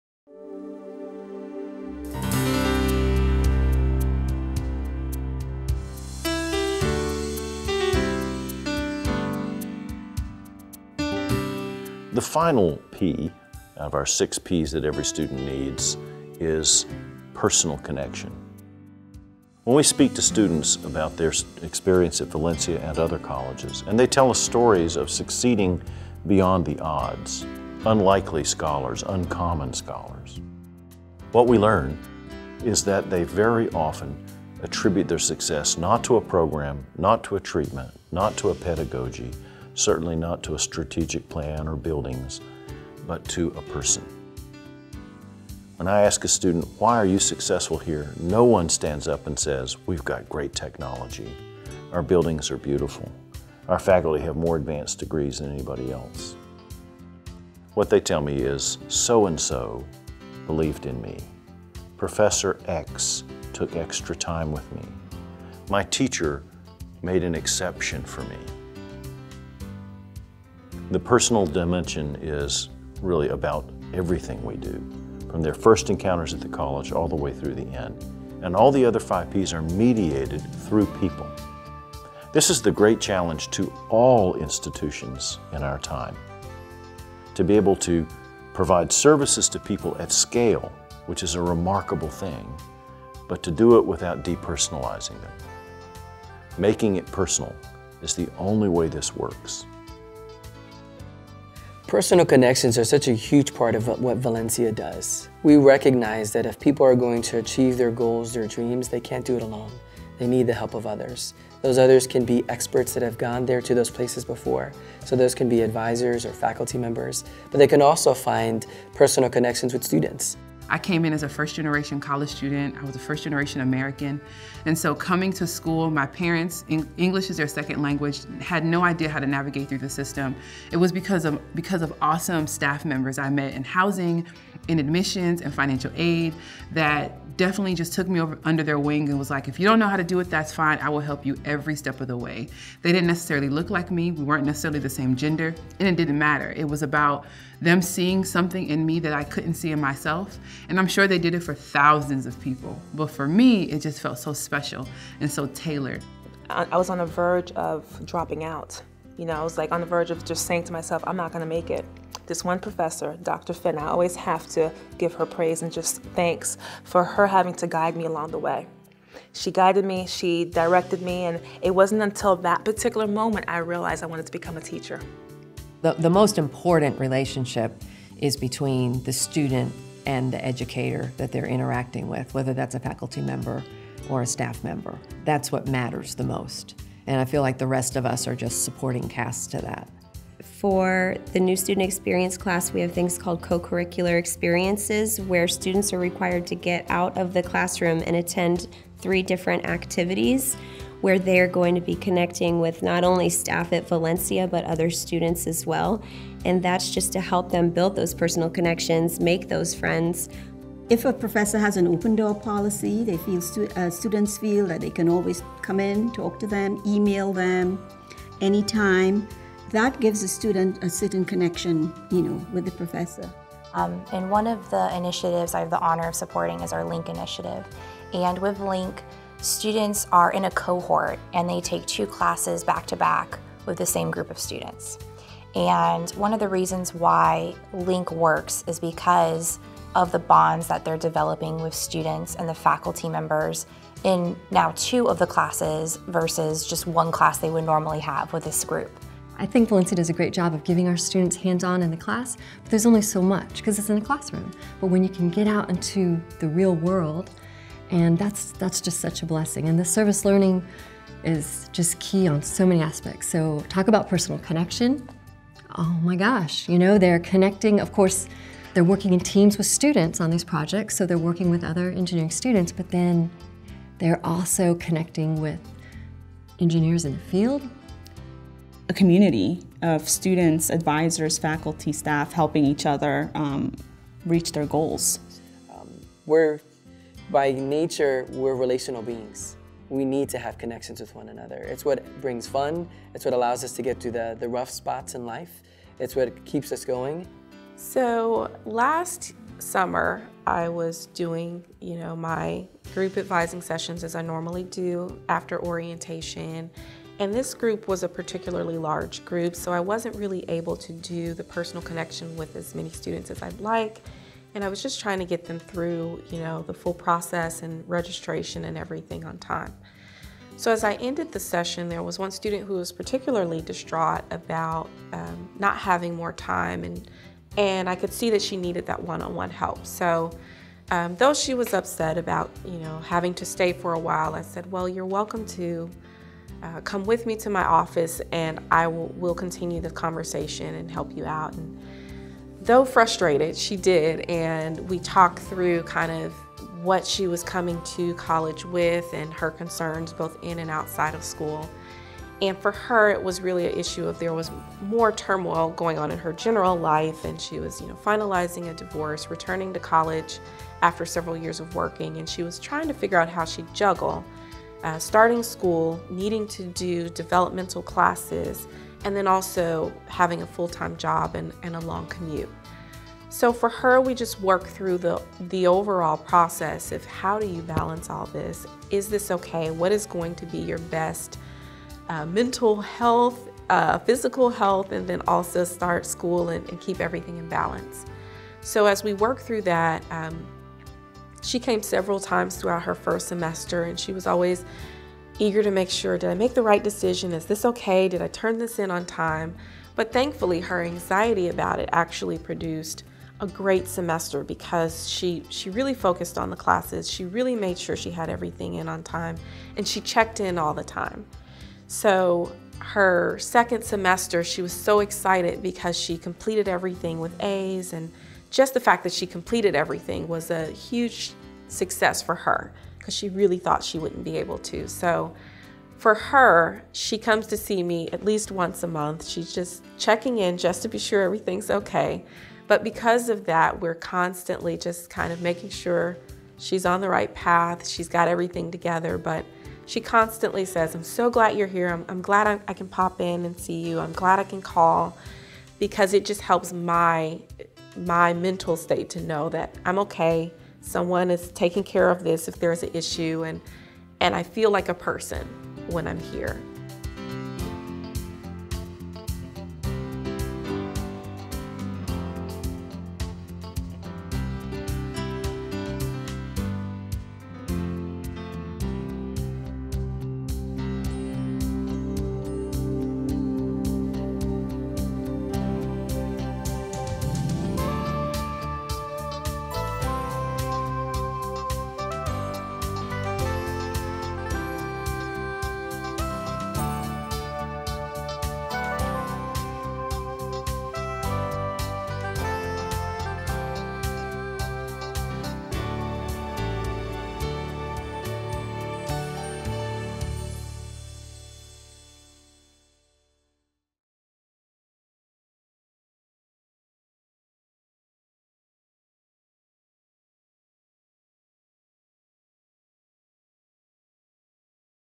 The Six Ps: Personal Connection
Valencia storytellers share the value of personal connection that has informed the design of the New Student Experience. Featured are Jonathan Hernandez, Lorie Bellot, Claudine Bentham, Joyce Romano, Heather Trees, Lois Crichlow, Robyn Brighton, Deb Hall, Micheline Maalouf and Ayanna Collins.